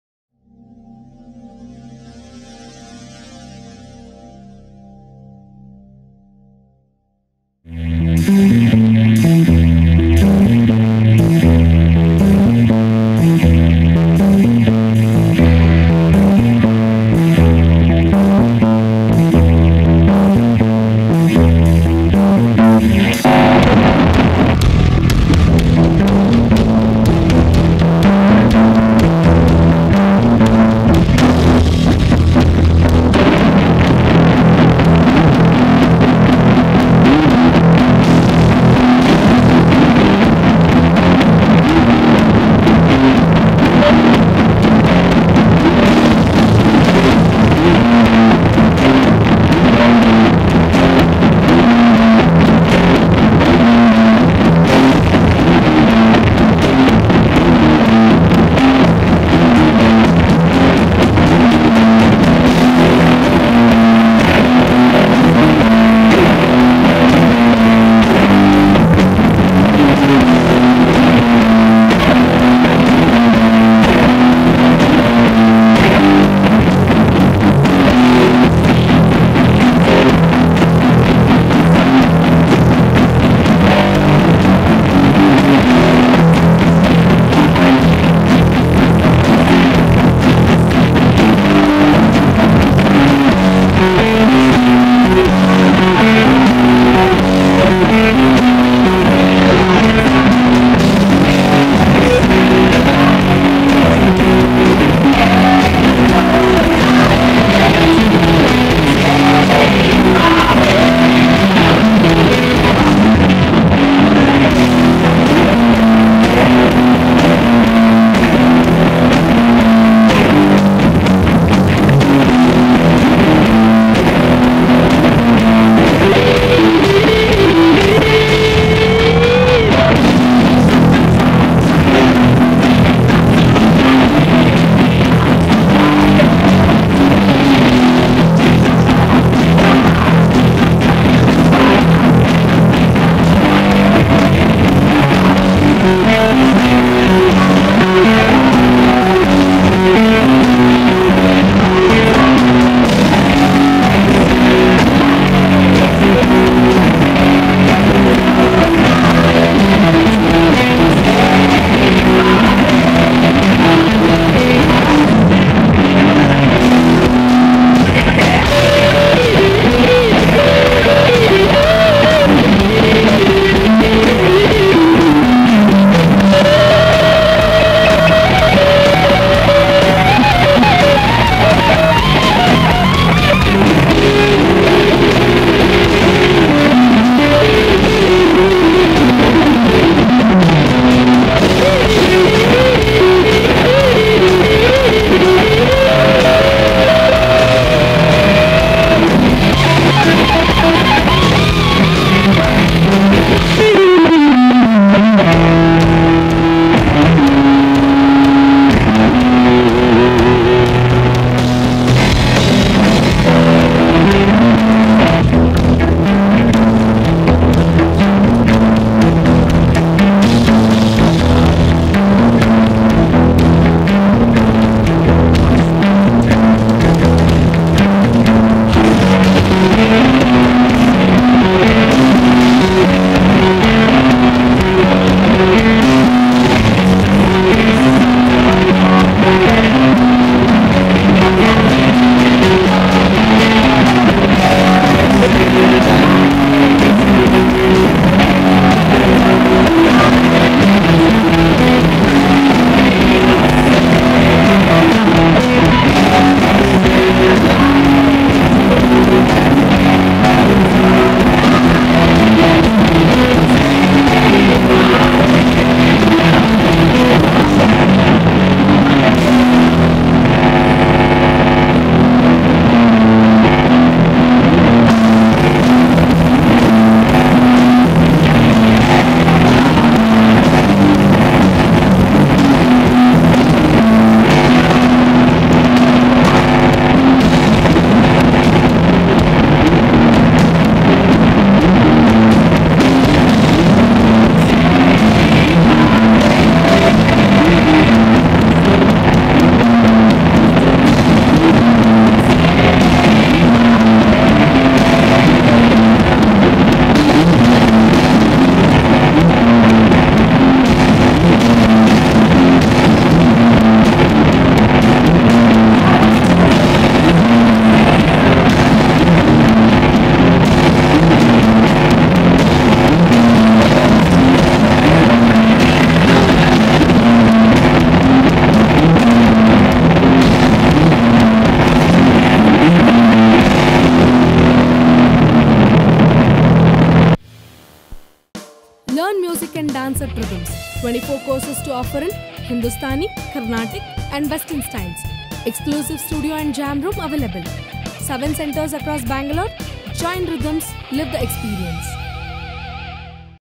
PRATHIBHANKURA ◦ 3 - 3 -24 ◦ GUITAR
Rhythms College of Fine arts
Presents

"Prathibhankura"
Rising Stars Talent Show

Guitar  Performance